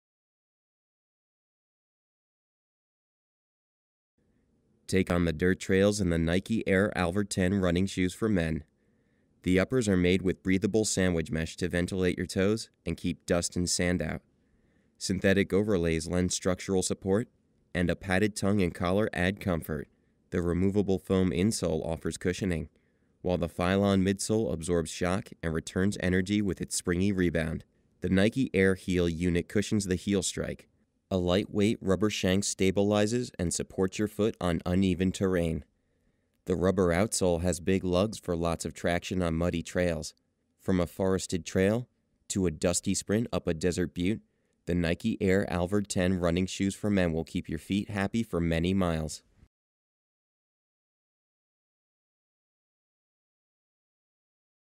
Nike Men's Air Alvord 10 Running Shoes
Watch our Nike Men's Air Alvord 10 Running Shoes Video and learn more about recommended features.